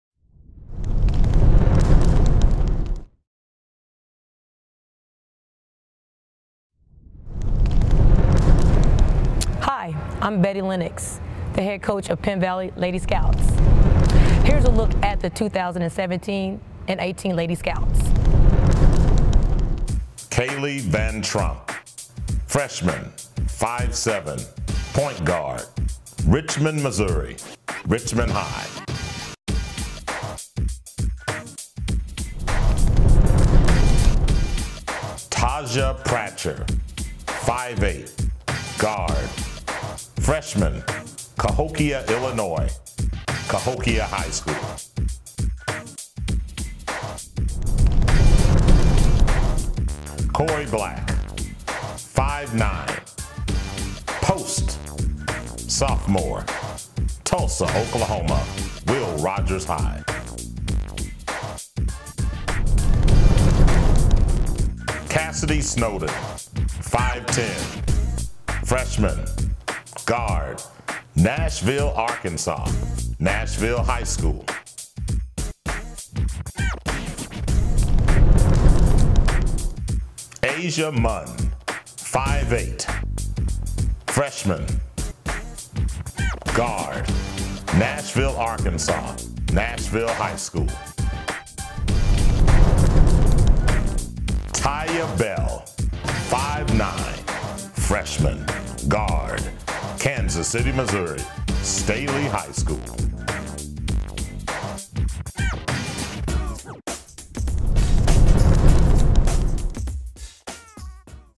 Hot Winter Nights Promotional video for Penn Valley women's basketball team 2017/18
Promotional video for Penn Valley women's basketball team 2017/18
Produced, Directed, shot and edited by Keith Jackson
Video samples of 3N1 Productions LLC_Keith Jackson, KCMO